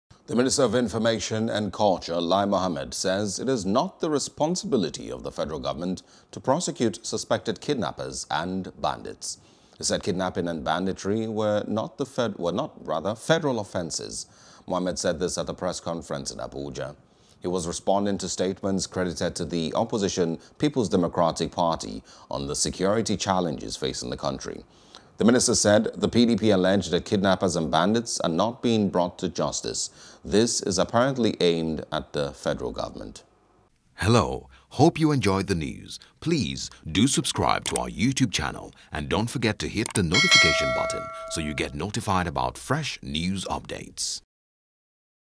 Prosecuting Kidnappers, Bandits Is Not FG’s Job —Lai Mohammed | NEWS
The Minister of Information and Culture,  Lai Mohammed, says it was not the responsibility of the Federal Government to prosecute suspected kidnappers and bandits.